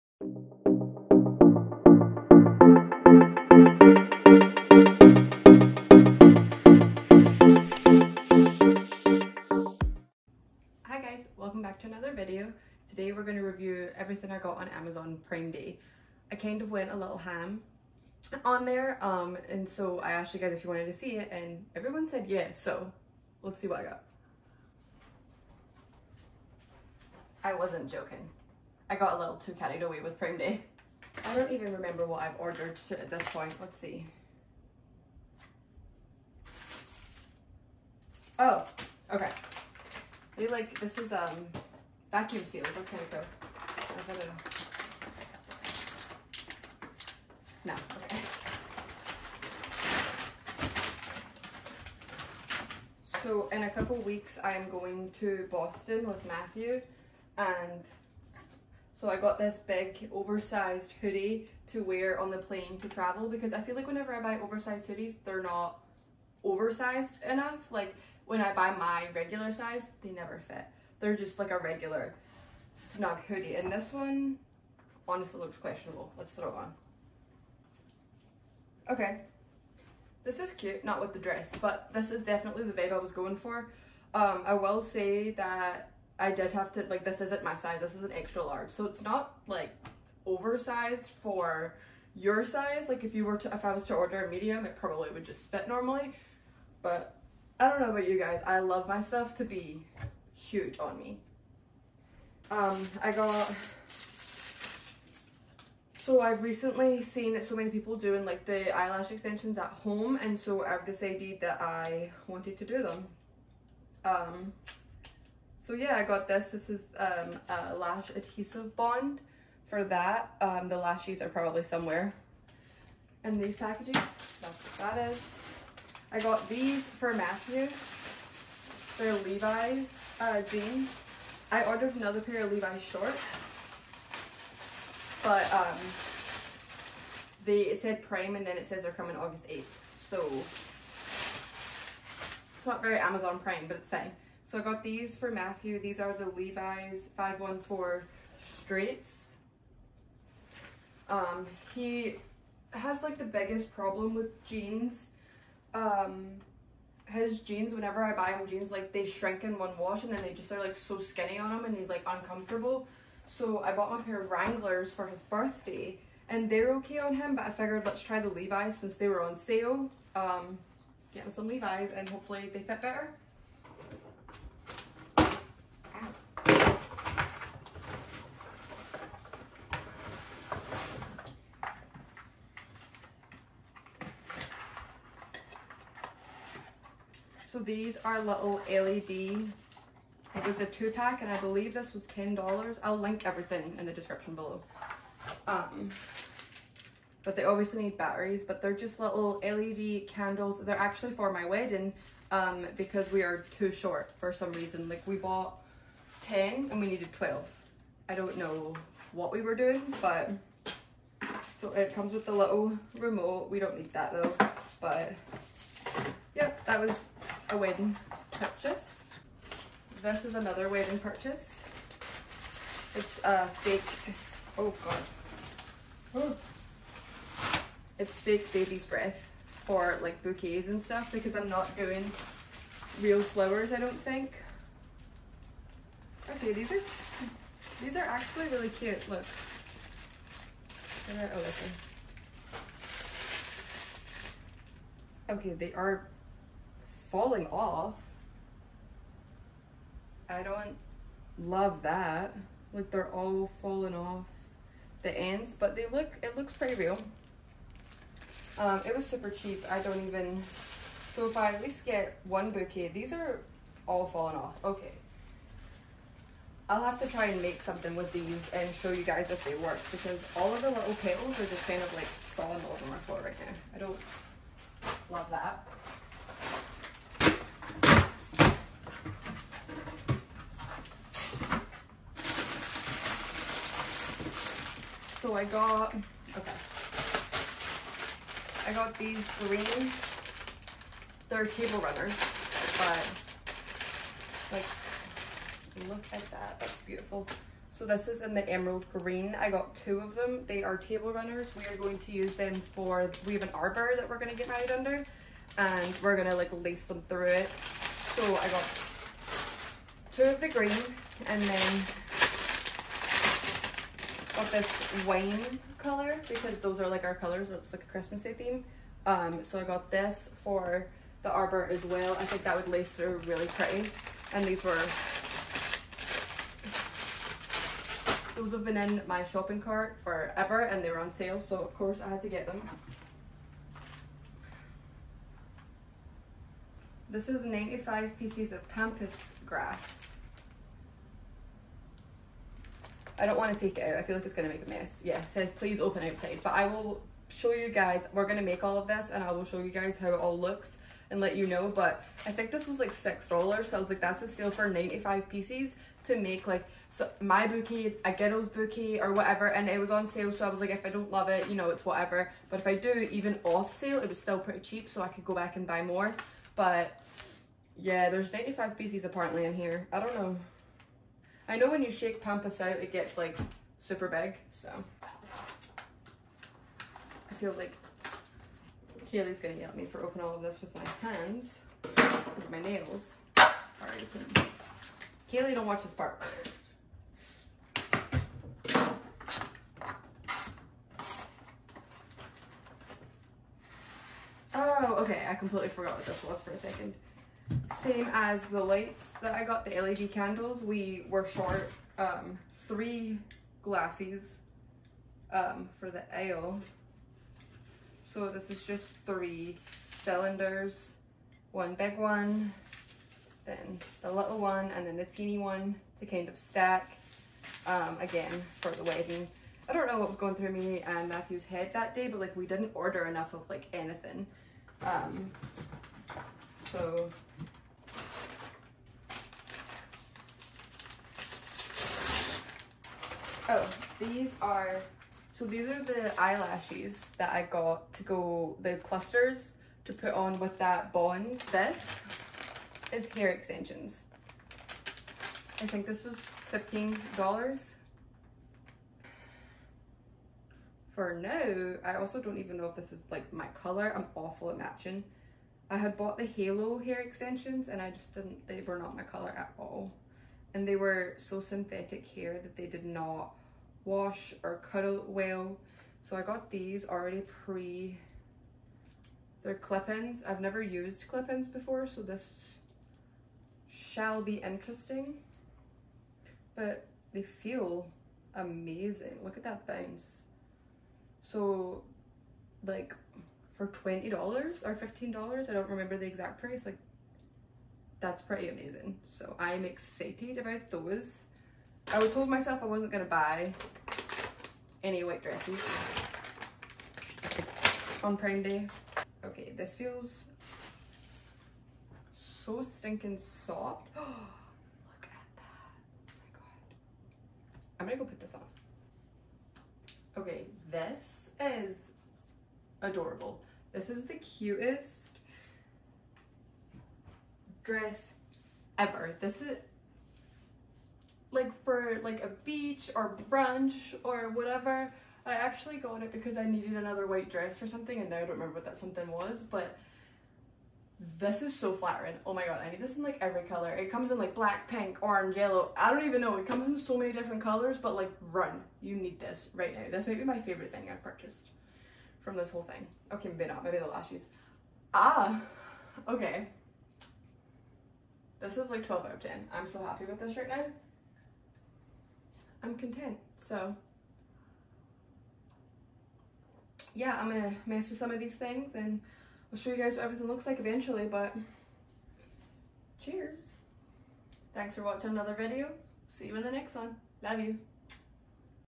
Amazon Prime Day Haul
Everything I got on Amazon Prime Day!!!

Hoodie is an XL

Lash adhesive bond

Matthew’s Mens Levi’s

LED candles

Baby’s breath

Table runners

Pampas grass

Cylinder glasses

Lash clusters

$20 hair extensions!!!

White dress!